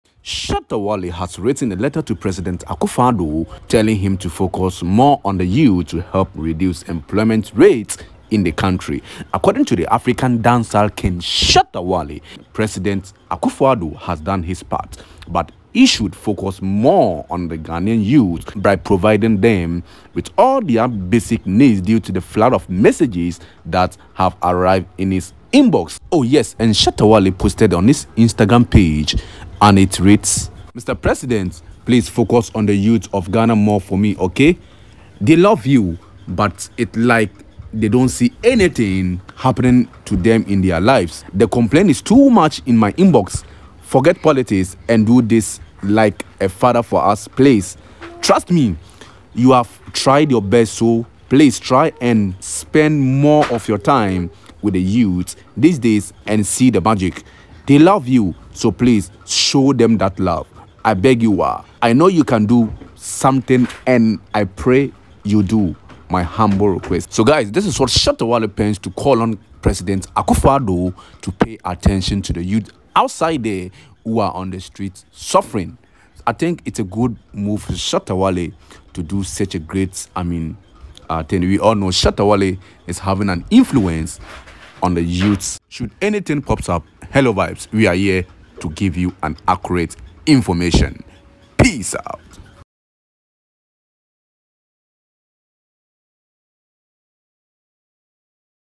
Forget Politics & Help The Crying Youths - Shatta Wale Writes A Letter To Prez. Akufo-Addo
Shatta Wale Writes A Letter To President Akufo-Addo To Focus More On The  Ghanaian Youths